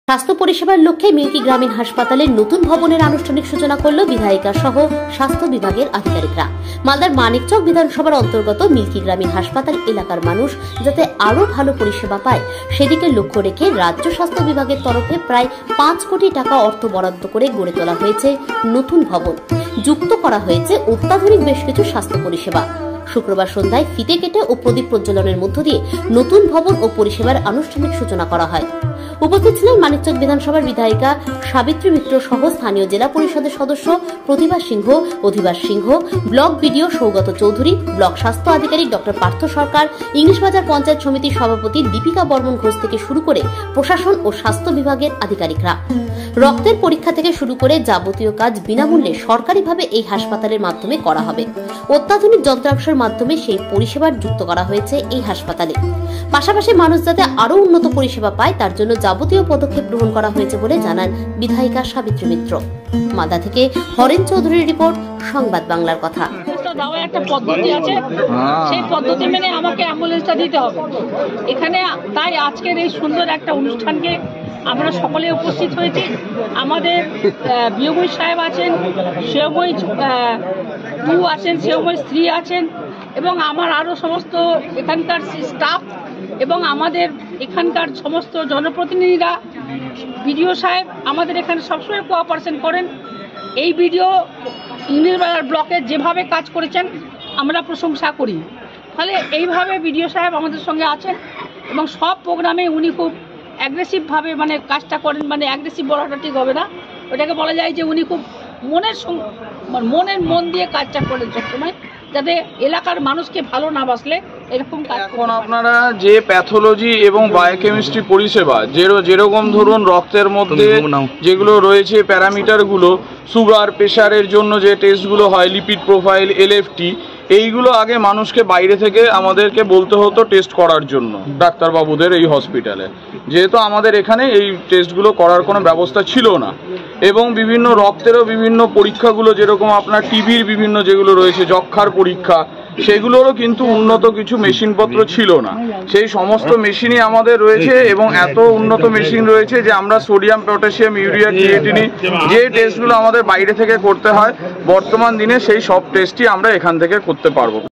স্বাস্থ্যপরিসেবার লক্ষ্যে মিল্কি গ্রামীণ হাসপাতালে নতুন ভবনের আনুষ্ঠানিক সূচনা
স্বাস্থ্যপরিসেবার লক্ষ্যে মিল্কি গ্রামীণ হাসপাতালে নতুন ভবনের আনুষ্ঠানিক সূচনা করলো বিধায়িকা সহ স্বাস্থ্য বিভাগের আধিকারিকরা। মালদার মানিকচক বিধানসভার অন্তর্গত মিলকি গ্রামীণ হাসপাতাল। এলাকার মানুষ যাতে আরো ভালো পরিষেবা পায় সেদিকে লক্ষ্য রেখে রাজ্য স্বাস্থ্য বিভাগের তরফে প্রায় পাঁচ কোটি টাকা অর্থ বরাদ্দ করে গড়ে তোলা হয়েছে নতুন ভবন যুক্ত করা হয়েছে অত্যাধুনিক বেশ কিছু স্বাস্থ্য পরিষেবা। শুক্রবার সন্ধ্যায় ফ্রিতে কেটে ও প্রদীপ পরিচালন এর মধ্য দিয়ে নতুন ভবন ও পরিষেবার আনুষ্ঠানিক সূচনা করা হয়। উপস্থিত ছিলেন মানিকচক বিধানসভার বিধায়িকার সাবিত্রী মিত্র সহ স্থানীয় জেলা পরিষদ সদস্য , প্রতিভা সিংহ,অধিবাস সিংহ,ব্লক বিডিও সৌগত চৌধুরী, ব্লক স্বাস্থ্য আধিকারিক ডঃ পার্থ সরকার, ইংলিশ বাজার পঞ্চায়েত সমিতির সভাপতি লিপিকা বর্মন ঘোষ থেকে শুরু করে প্রশাসন ও স্বাস্থ্য বিভাগের আধিকারিকরা। রক্তের পরীক্ষা থেকে শুরু করে যাবতীয় কাজ বিনামূল্যে সরকারিভাবে এই হাসপাতালের মাধ্যমে করা হবে। অত্যাধুনিক যন্ত্রাংশ মাধ্যমে সেই পরিষেবার যুক্ত করা হয়েছে এই হাসপাতালে। পাশাপাশি মানুষ যাতে আর উন্নত পরিষেবা পায় তার জন্য যাবতীয় পদক্ষেপ গ্রহণ করা হয়েছে বিলে জানান বিধায়িকা সবিত্রী মিত্র।